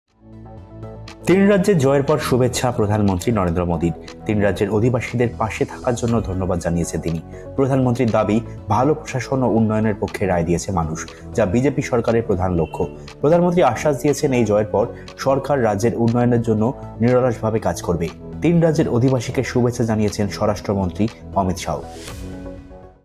PM Narendra Modi: জনতাই জনার্দন, তিন রাজ্যের জয়ের পর অভিনন্দন মোদীর, শুভেচ্ছা তেলঙ্গনাকেও
তিন রাজ্যের জয়ের পর ধন্যবাদ প্রধানমন্ত্রী নরেন্দ্র মোদীর। তিন রাজ্যের অধিবাসীদের পাশে থাকার জন্য ধন্যবাদ জানিয়েছেন তিনি। প্রধানমন্ত্রীর দাবি, ভাল প্রশাসন, উন্নয়নের পক্ষে রায় দিয়েছে মানুষ। যা বিজেপি সরকারের প্রধান লক্ষ্য।